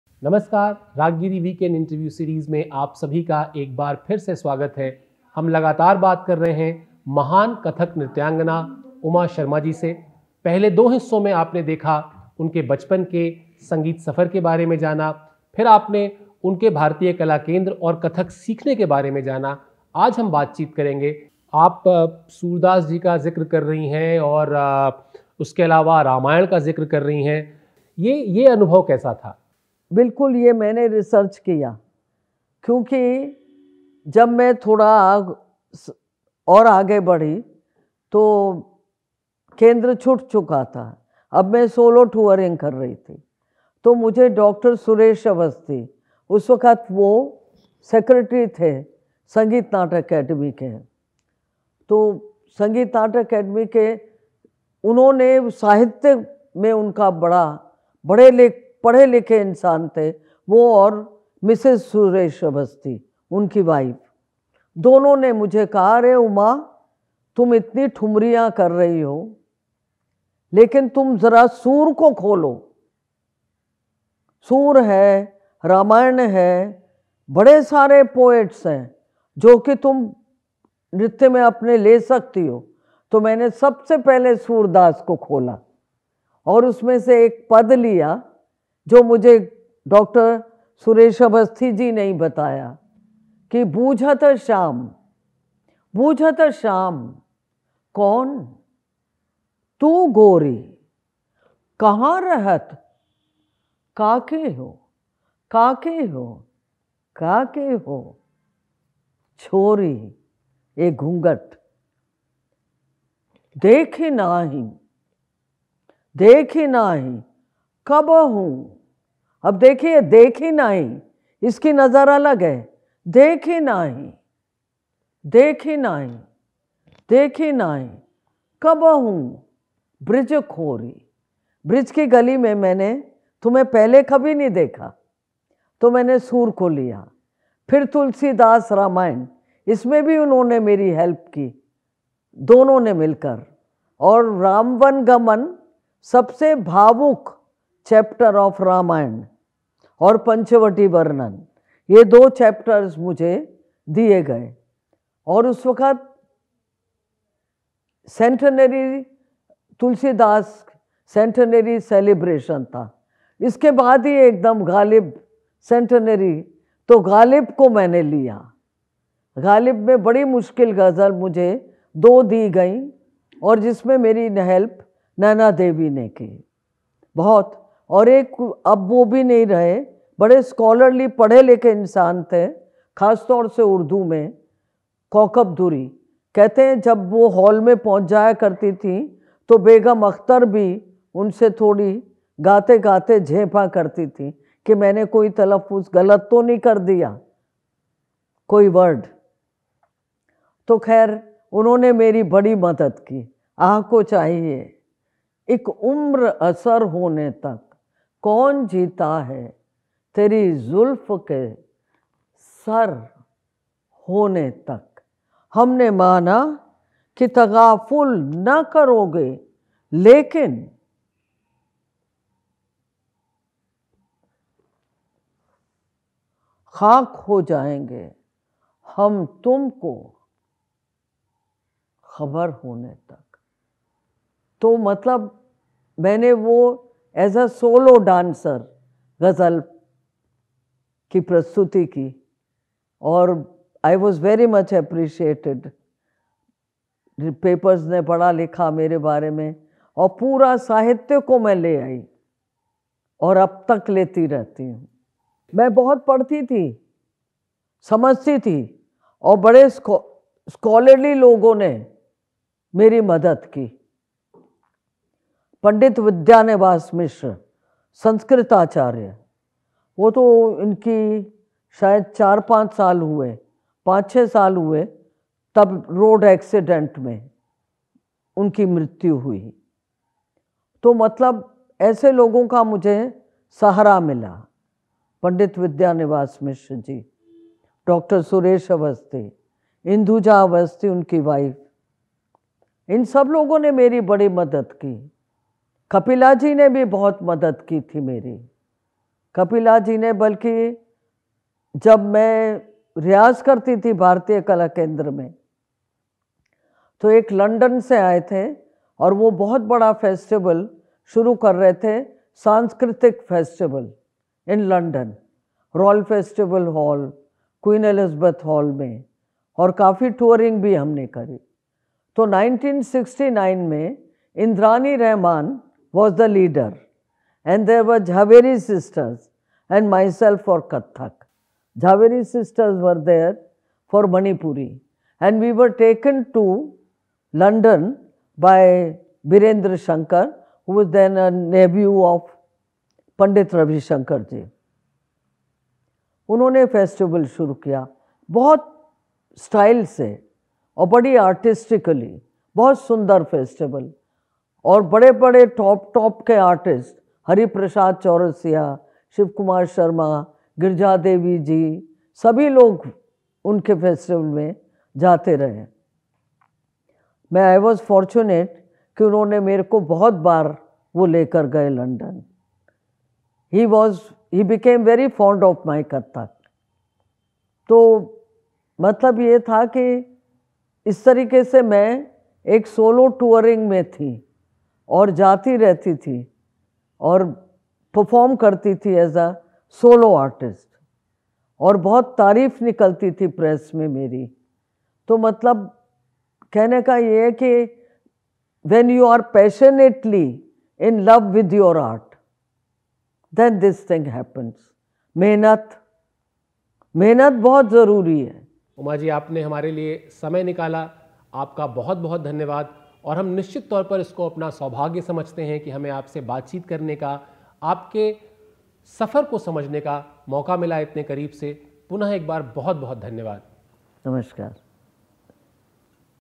Kathak Exponent Dr. Uma Sharma Exclusive Interview Part 03 💃🎭 Raaggiri
Watch an exclusive interview of Kathak exponent Dr. Uma Sharma part 03 on Raaggiri. Uma Sharma runs her own School of Music and Dance in the capital and has trained a whole new generation of younger dancers. However, veteran dance critic and scholar Sunil Kothari of New Delhi, has criticized her dance as always being very Bollywood oriented in nature. He has also accused her of misusing her connections with various government officials to gain awards and publicity. Uma hasn't commented on such allegations.

00:30 - Uma Sharma ji talking about surdas, ghalib and ramayana

Kathak Exponent Dr. Uma Sharma Career
After having learnt the presentation of traditional items, she has widened the repertoire of Kathak by composing new dance numbers and full length dance-dramas on a variety of themes. Her dance drama Stree (Woman), has been known its powerful thematic content and artistic presentation. As a one-woman exposition Stree Kathak gives emotive thrust in depicting the position of Woman down the centuries and her search for an independent identity.
Uma has performed all over the country and participated in many a national and international festivals. She has been on performance tours to USSR, New Zealand, Australia, USA, Canada, Middle East, Japan and China, both on invitation from organizations abroad and as a representative of the Department of Culture and the Indian Council for Cultural Relations.